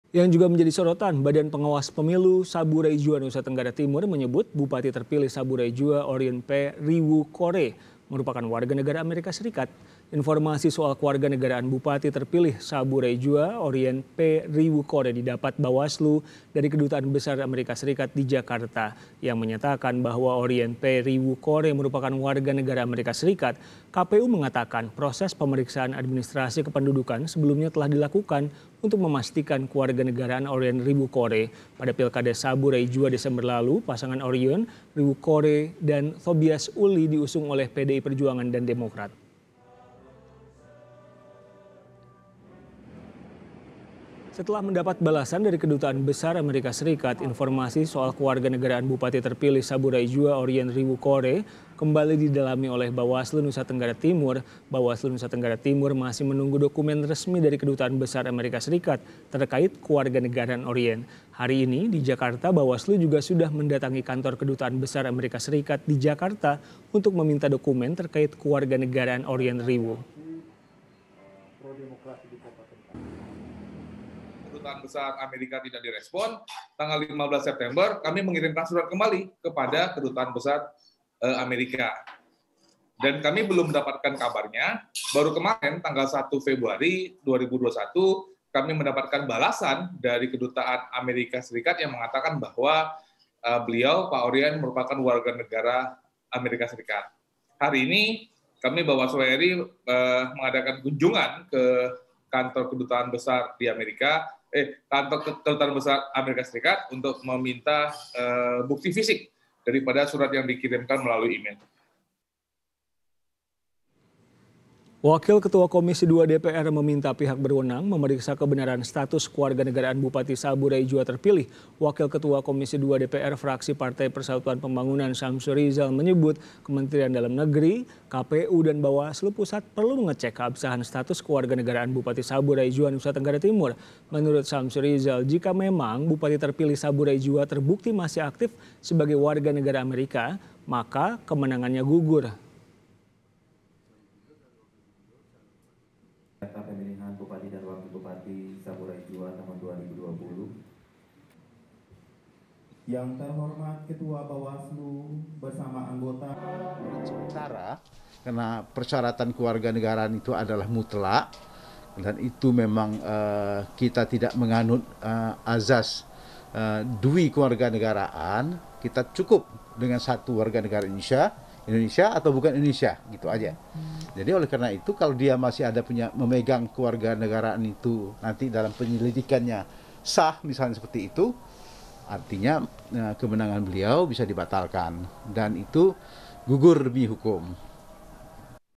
Jika Terbukti Bupati Terpilih Sabu Raijua Adalah WN Amerika, Maka Kemenangannya Gugur
NUSA TENGGARA TIMUR, KOMPAS.TV - Bawaslu Sabu Raijua, Nusa Tenggara Timur, menyebut Bupati terpilih Sabu Raijua, Orient P Riwu Kore, merupakan warga negara Amerika Serikat.

Informasi soal kewarganegaraan Bupati terpilih Sabu Raijua, Orient P Riwu Kore, didapat Bawaslu dari Kedutaan Besar Amerika Serikat di Jakarta yang menyatakan bahwa Orient P Riwu Kore, merupakan warga negara Amerika.

KPU mengatakan proses pemeriksaan administrasi kependudukan sebelumnya telah dilakukan untuk memastikan kewarganegaraan Orient Riwu Kore.

Pada Pilkada Sabu Raijua Desember lalu, pasangan Orient Riwukore dan Thobias Uly Diusung oleh PDIP dan Demokrat.

Setelah mendapat balasan dari Kedutaan Besar Amerikas Serikat, informasi soal kewarganeragaan bupati terpilih Sabu Raijua, Orient Riwu Kore, kembali didalami oleh Bawaslu Nusa Tenggara Timur.

Bawaslu Nusa Tenggara Timur Masih menunggu dokumen resmi dari Kedutaan Besar Amerika Serikat terkait kewarnegaraan Orient.

Hari ini (03/02) di Jakarta, Bawaslu juga sudah mendatangi kantor Kedutaan Besar Amerika Serikat di Jakarta untuk meminta dokumen terkait kewarganegaraan Orient Riwu.

Semenatara itu Wakil Ketua Komisi II DPR RI, meminta, pihak berwenang, untuk memeriksa kebenaran status kewarganegaraan, Bupati Sabu Raijua terpilih.

Wakil Ketua Komisi II DPR Fraksi Partai Persatuan Pembangunan, Syamsurizal, menyebut, Kemendagri, KPU, dan Bawaslu pusat, perlu mengecek, keabsahan status kewarganegaraan, Bupati Sabu Raijua, Nusa Tenggara Timur.

Menurut Syamsurizal, jika memang Bupati terpilih Sabu Raijua terbukti masih aktif sebagai Warga Negara Amerika, maka kemenangannya, gugur.